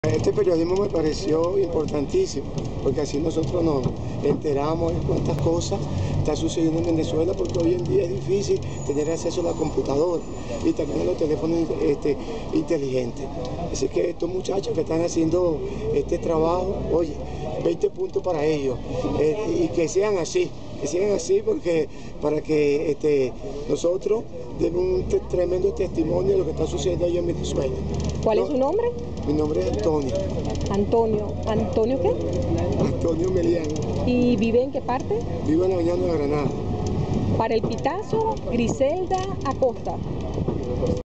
Oyentes del BusTV agradecidos por esta iniciativa informativa
El Pitazo en la Calle salió junto a los reporteros del BusTV del recorrido El Paraíso- La Vega a conocer la opinión de sus pasajeros. Desde aplausos hasta bendiciones se escucharon. Palabras de aliento para continuar en esta labor informativa relajada, amigable que no representa  ningún costo para los oyentes. Antonio Meleán dejó su opinión y celebró que existan jóvenes informando porque actualmente no todos puedn disfrutar del Derecho de Información.